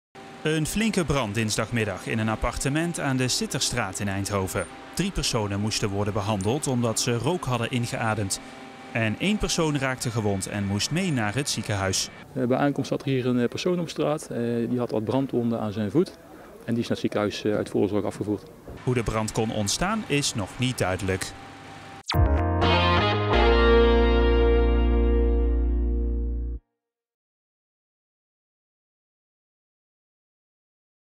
Woningbrand in De Sitterstraat
Een flinke brand dinsdagmiddag in een appartement aan De Sitterstraat in Eindhoven.

Drie personen moesten worden behandeld, omdat ze rook hadden ingeademd. Eén persoon raakte gewond en moest mee naar het ziekenhuis.

Hoe de brand kon ontstaan, is nog niet duidelijk.